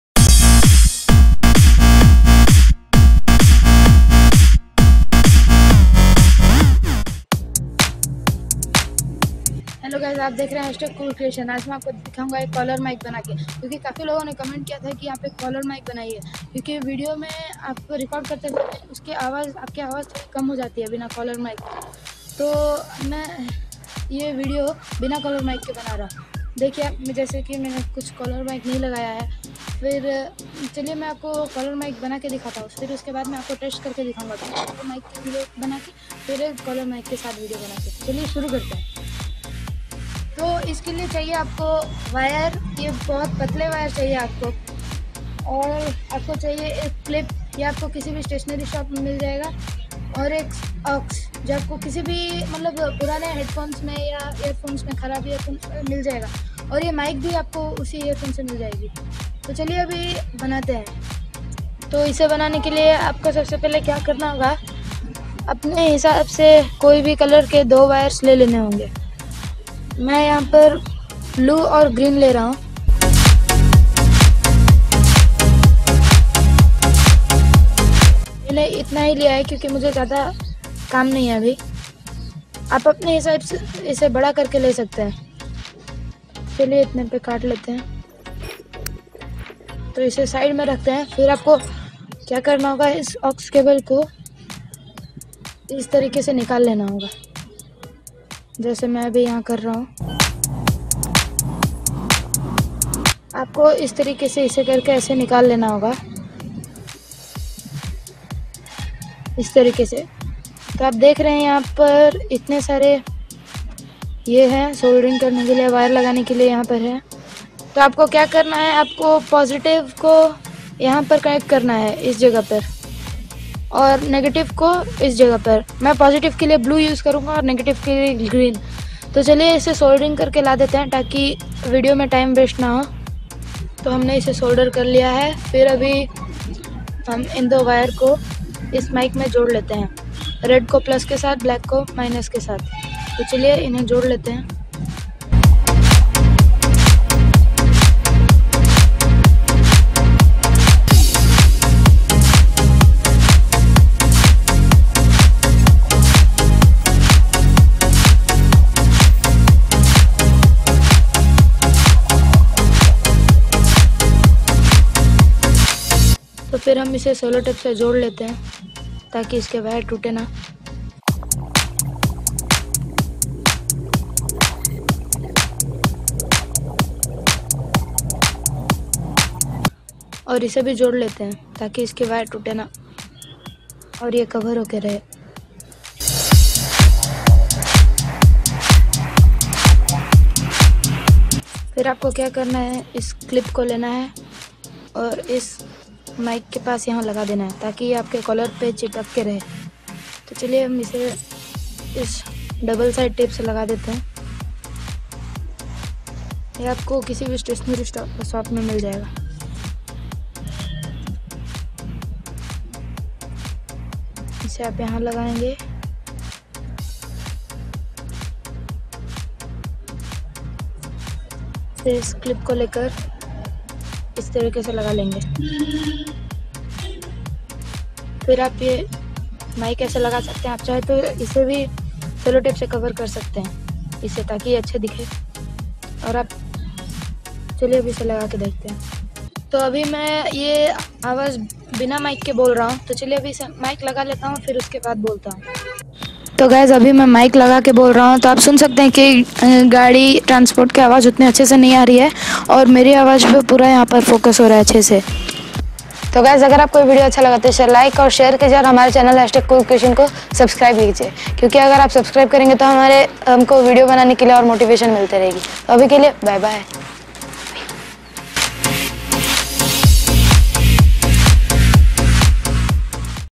How to make a good quality collar mic with an old broken ear phone||cool creation
hello guys i am going to show you how to make a good quality collar or lapel mic with an old broken earphone for recording good sound/audio quality in a video and to make video more attractive.